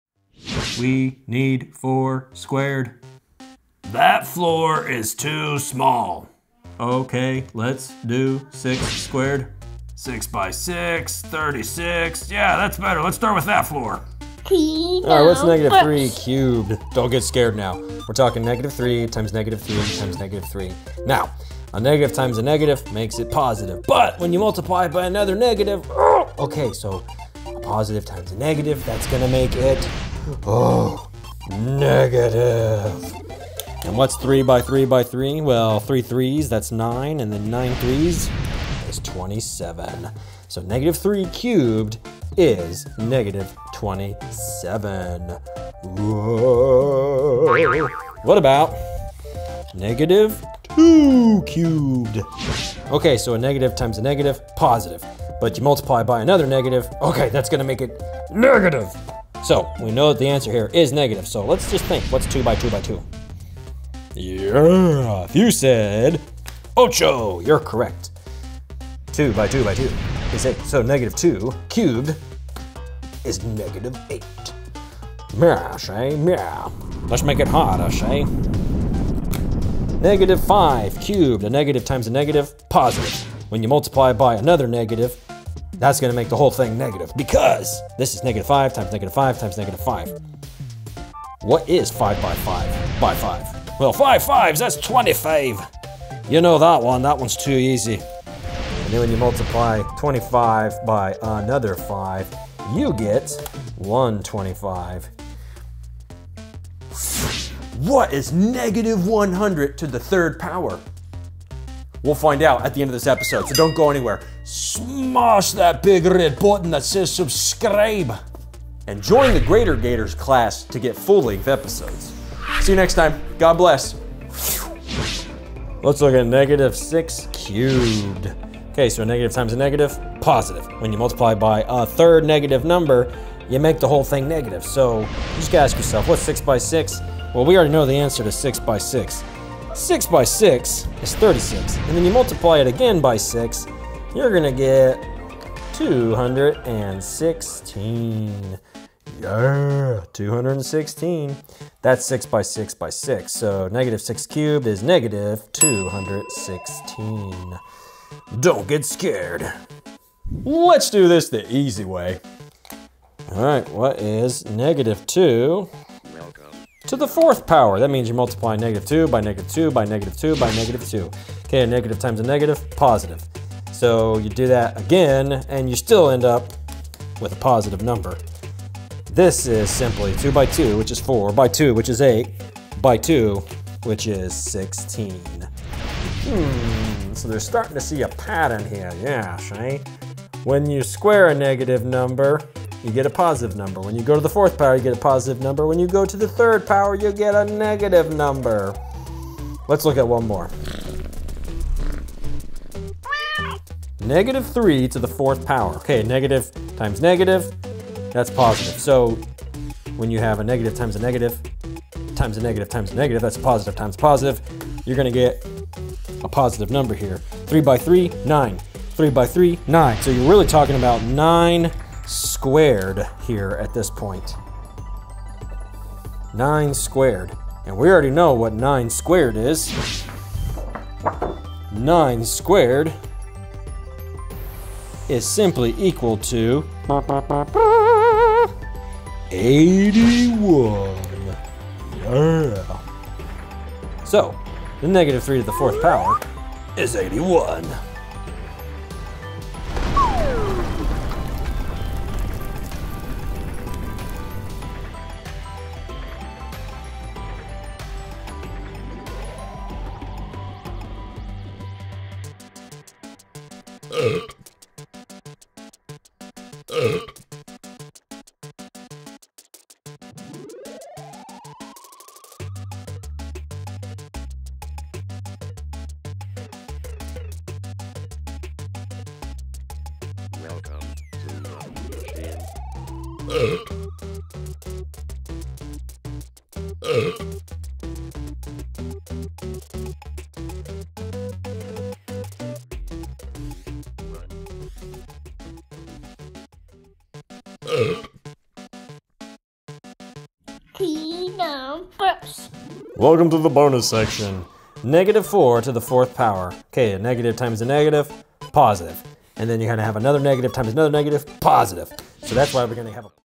Exponents: The Easy Way to Look at Cubes | 5th & 6th Grade Math
In this introductory video, we are going to look at the wonderful world of exponents. What the deuce?! This is a shortcut to do multiplication, the kind of multiplication that's really repetitive. Grab a square! You might want a visual aid to help really cement this concept before we start learning about the order of operations. Don’t get scared! Viewer beware! This video has… Burps. But more importantly, it's a solid intro into the world of exponents.

Job 38:4-5